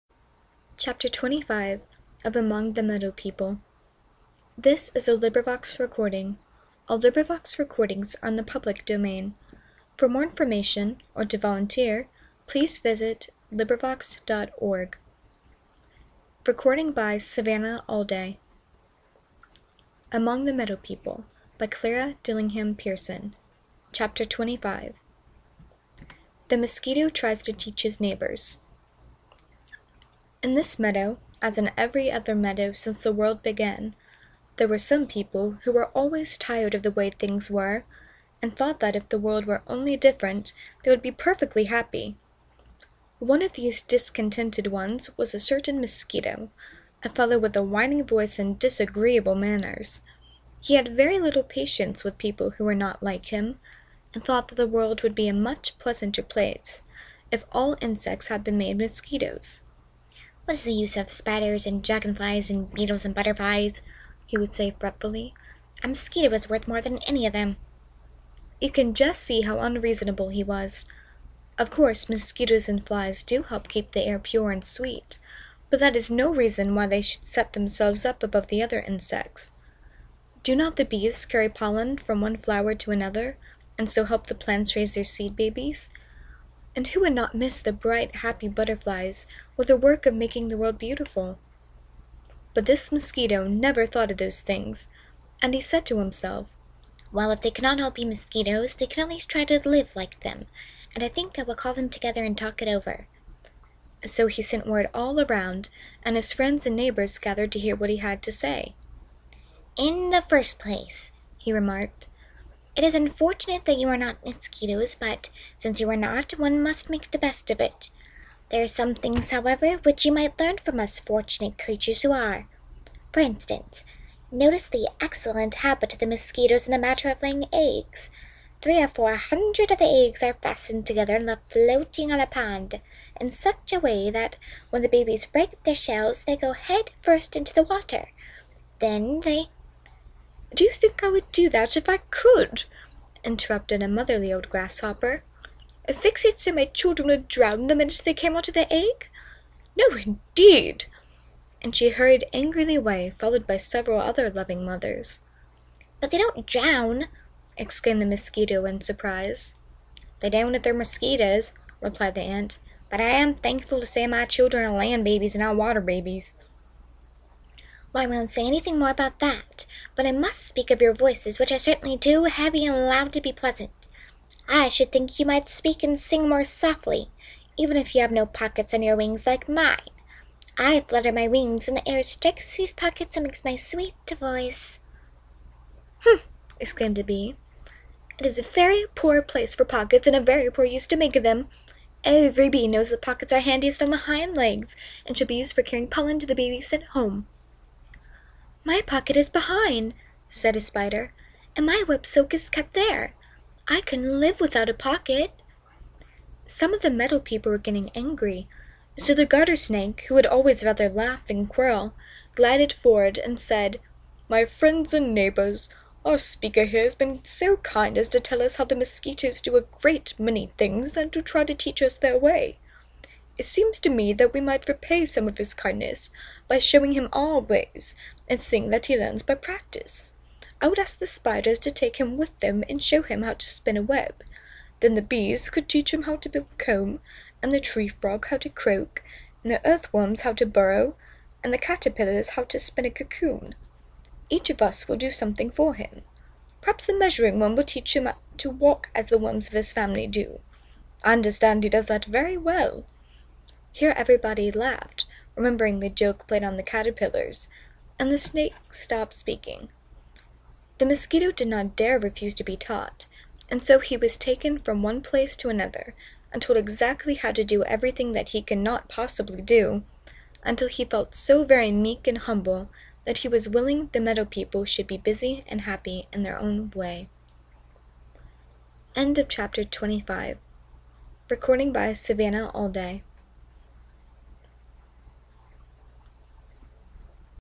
Among the Meadow People: The Mosquito Tries to Teach His Neighbors
Book Title: Among the Meadow People
Chapter Title: The Mosquito Tries to Teach His Neighbors
Author: Clara Dillingham Pierson
Performer: LibriVox - Savannah